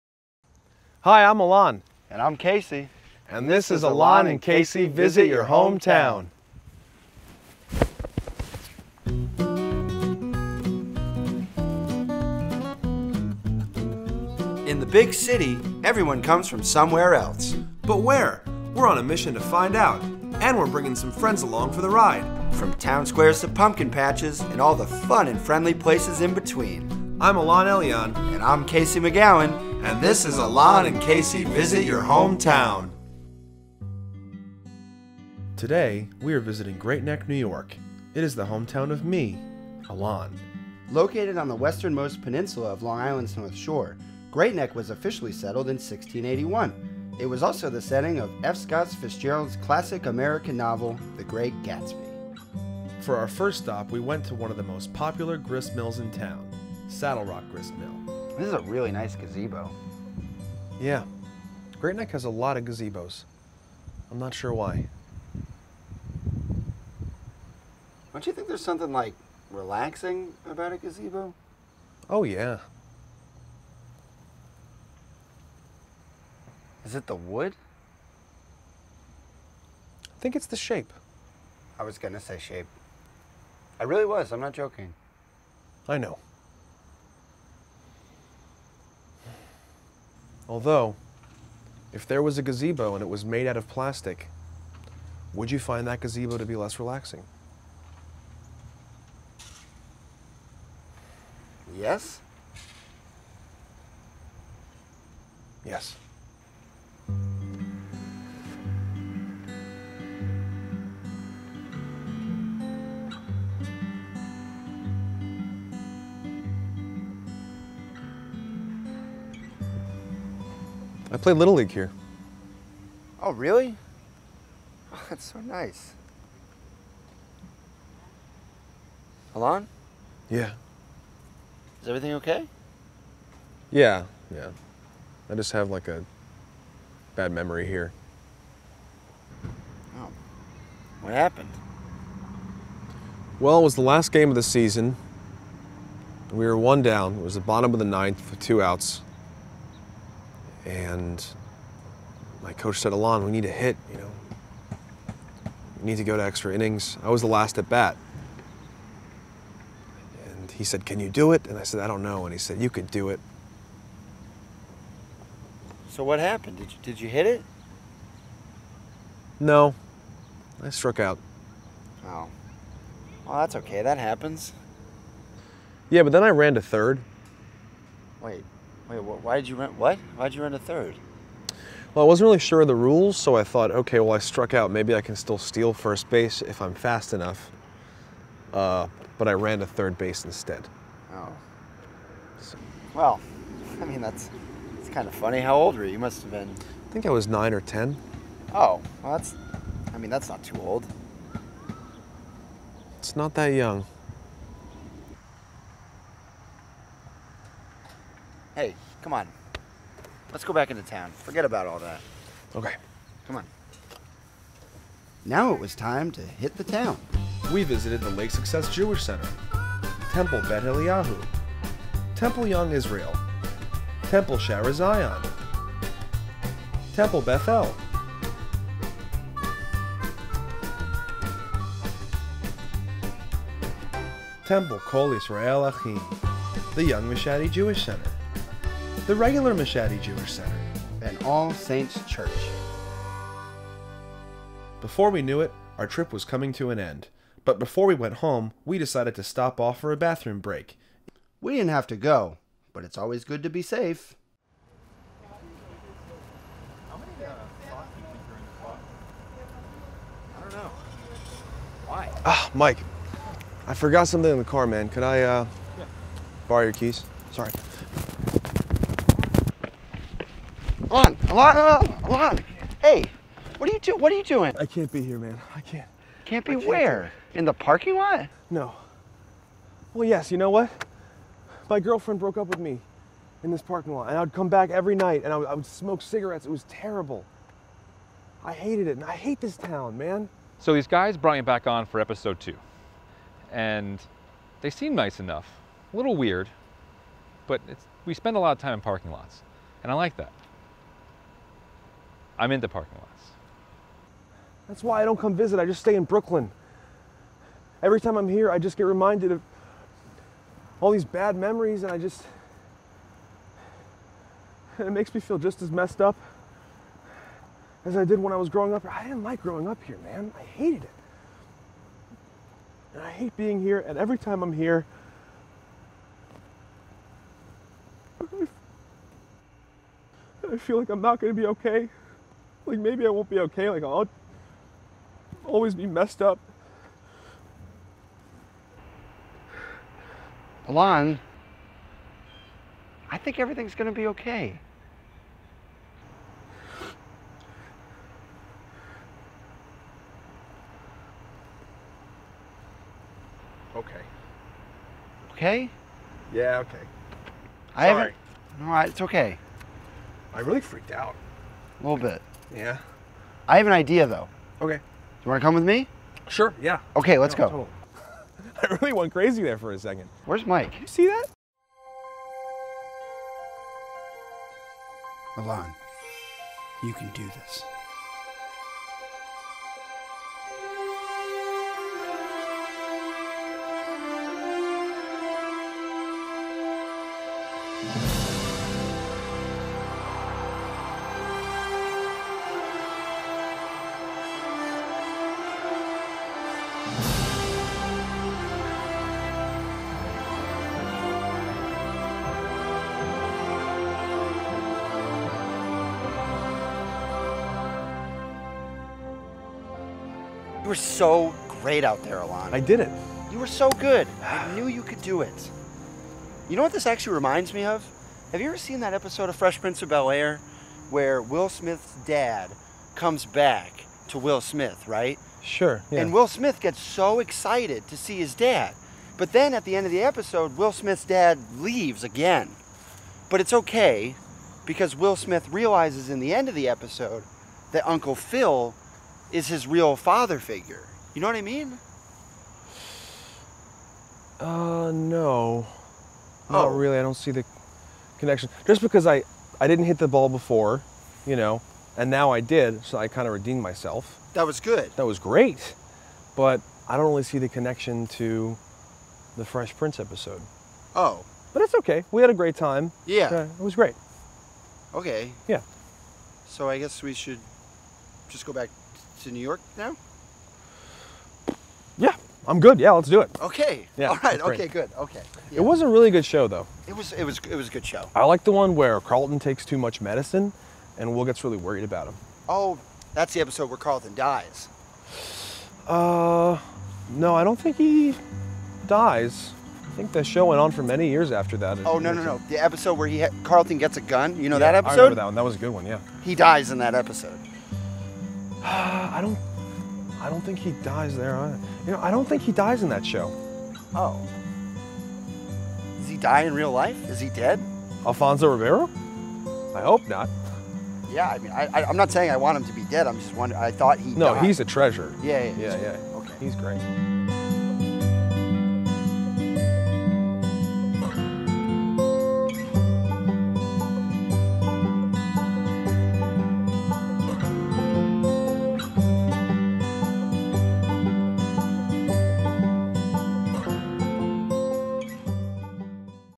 Alon & Casey Visit Your Hometown - Episode 2: Great Neck, New York
Alon and Casey visit Alon's hometown of Great Neck, New York. What starts as a nice and relaxing weekend, ends that way too!